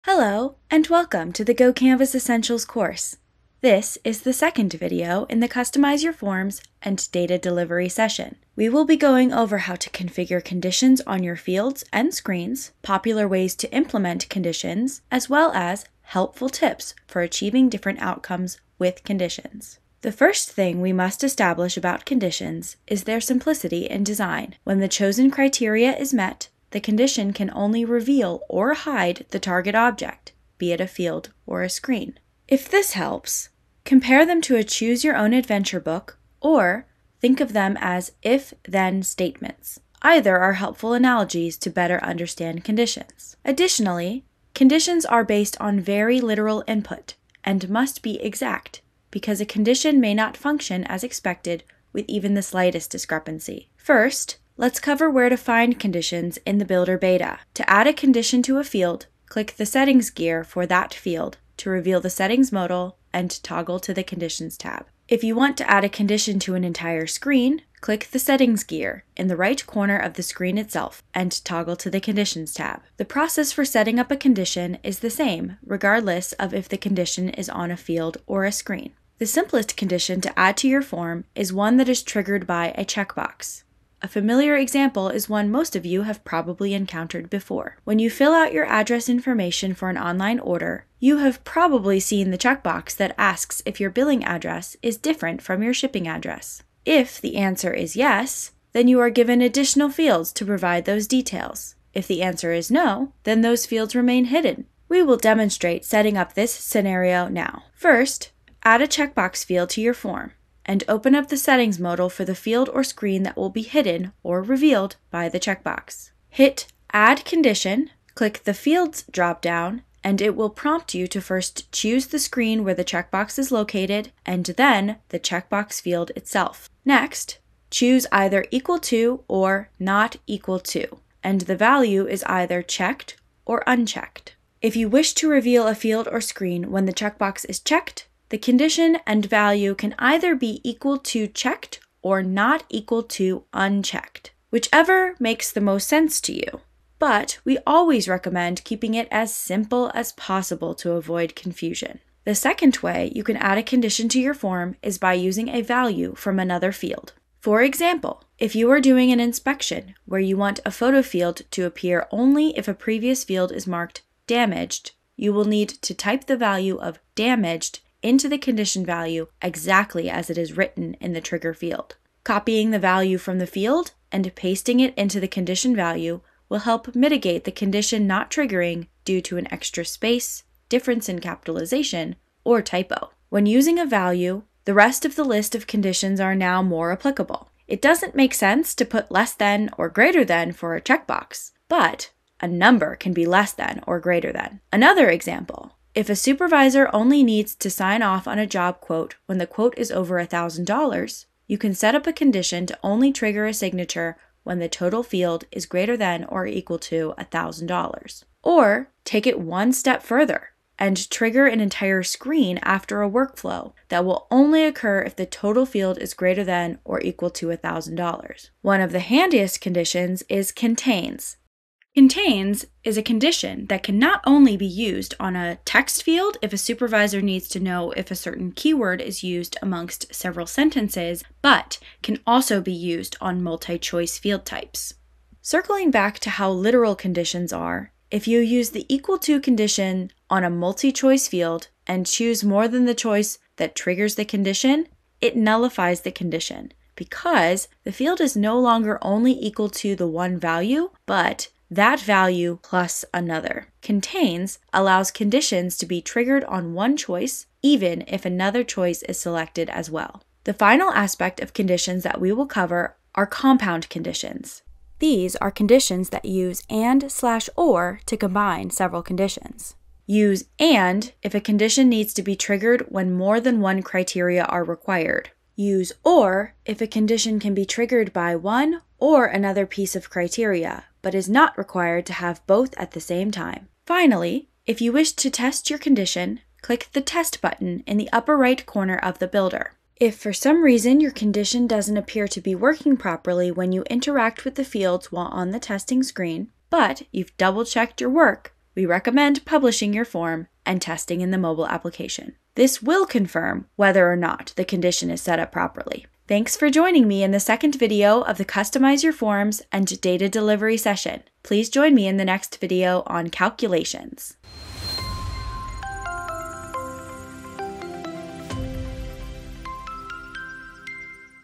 Customize Your Forms with Conditions
GoCanvas Essentials Course: Customize Your Forms & Data Delivery

This video is all about setting up and utilizing conditions within your GoCanvas forms.